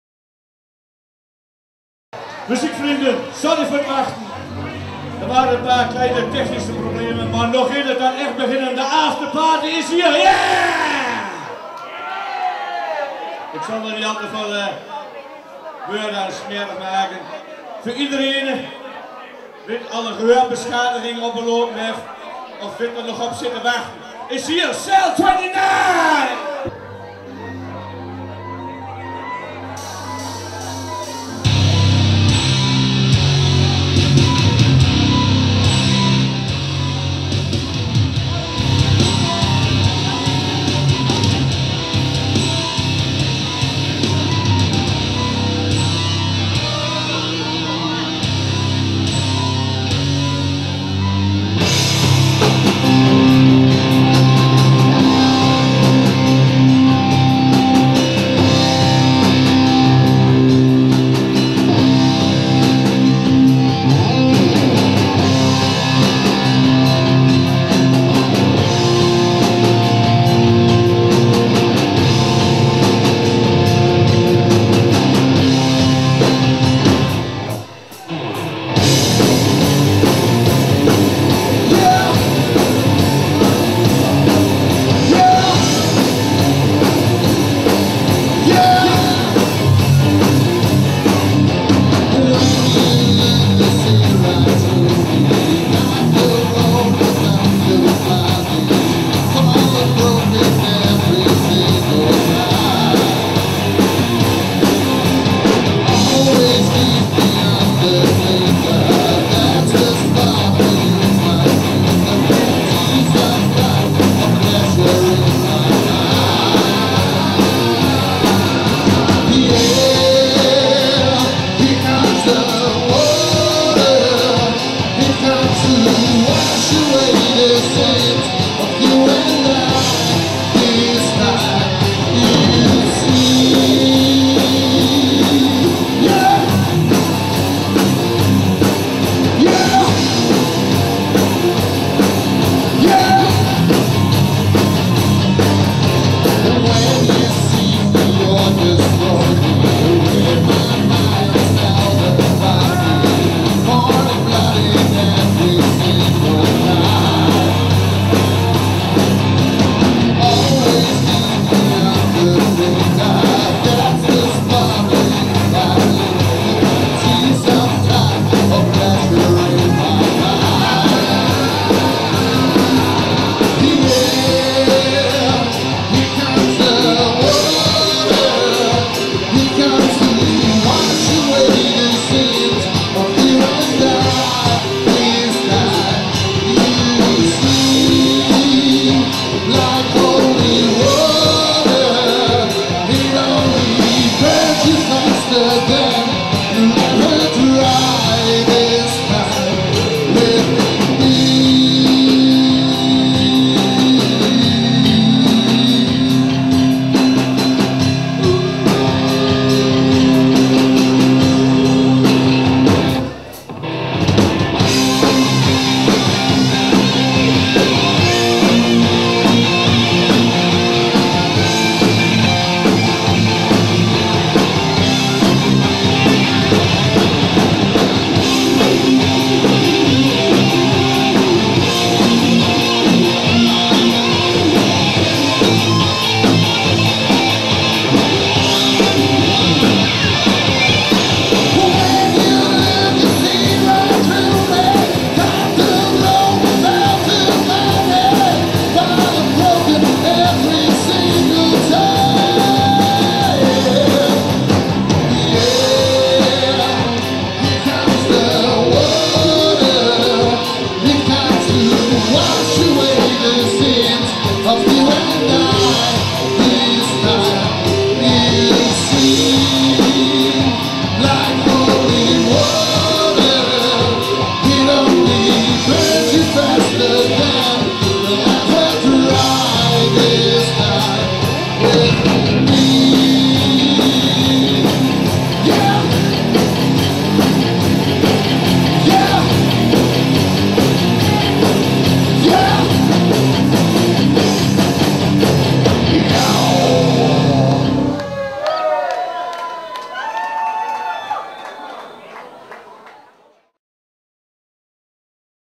Rock of Ages 2014 - CELL 29
Optreden van de band CELL 29 in Oerkroeg Schiller Aalten tijdens de 11e Rock of Ages.